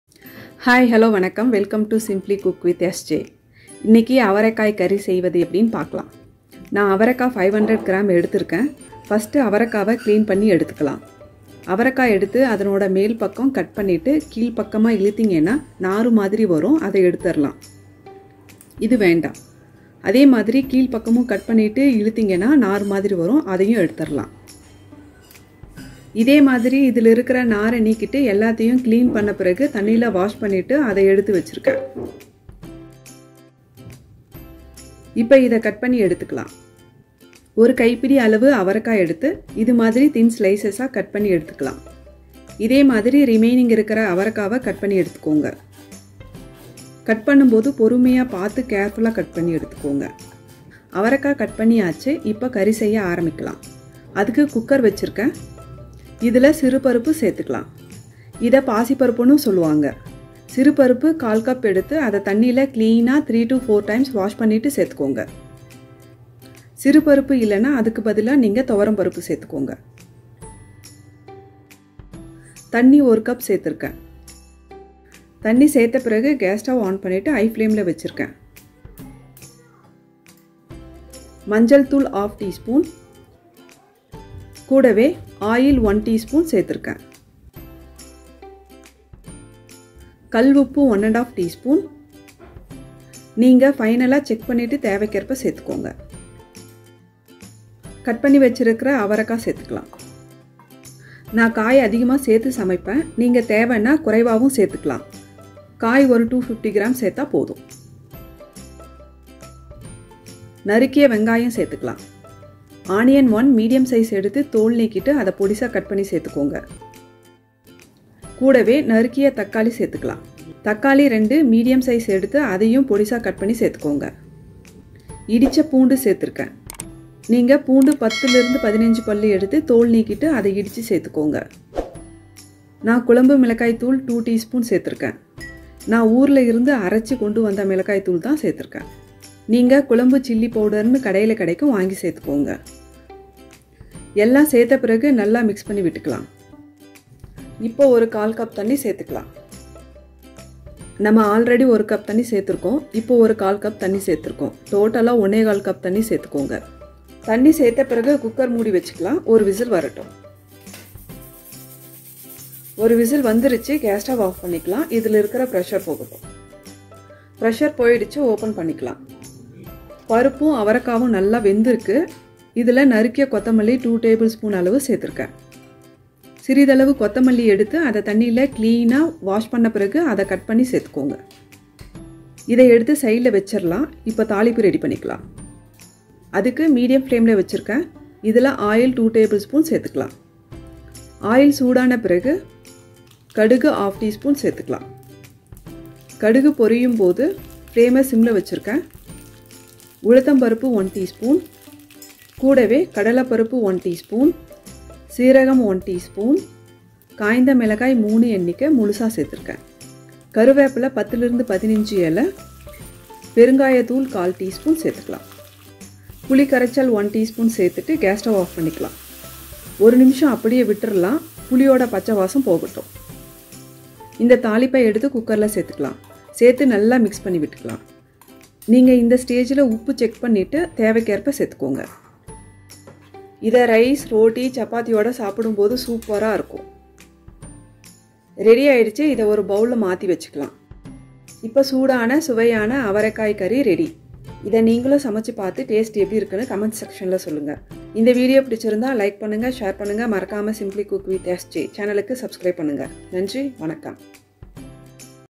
Avarakkai Curry - Side dish for Rice Roti Chapathi | Broad Bean curry
Avarakkai curry recipe, broad beans cooked in onion tomato base, a simple curry for south Indian lunch.

This is cooked with Moong Dal, tamarind paste and combined with spices to give a tangy flavour

Avarakkai Curry - Side dish for Rice Roti Chapathi | Bread Bean curry

To buy Cooker